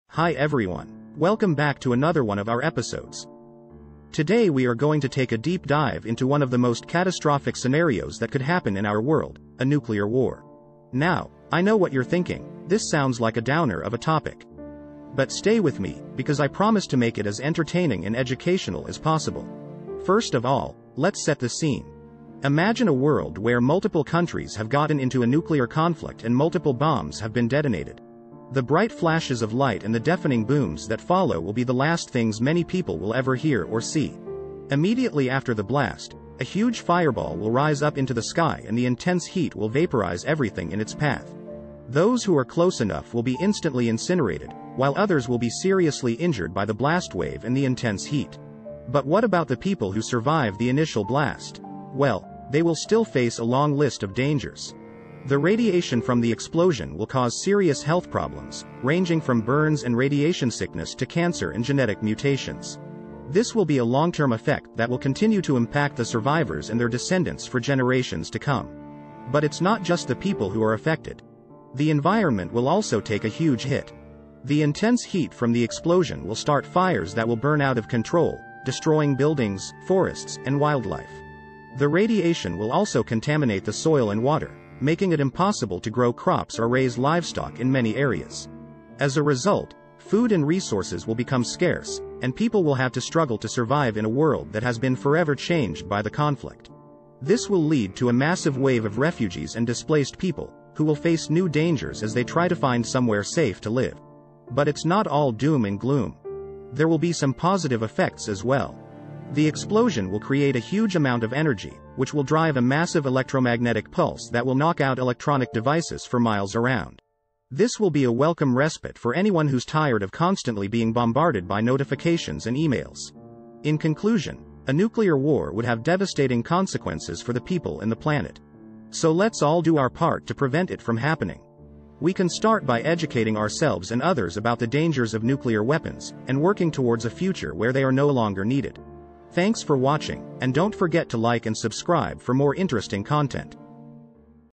The Aftermath of a Nuclear War: A Reality Check
Join us as we explore the catastrophic aftermath of a hypothetical nuclear war. From the initial blast to the long-term consequences for people and the environment, we'll take a look at what life could be like in a post-nuclear world. This video will leave you with a deeper understanding of the dangers of nuclear weapons and the importance of preventing them from ever being used.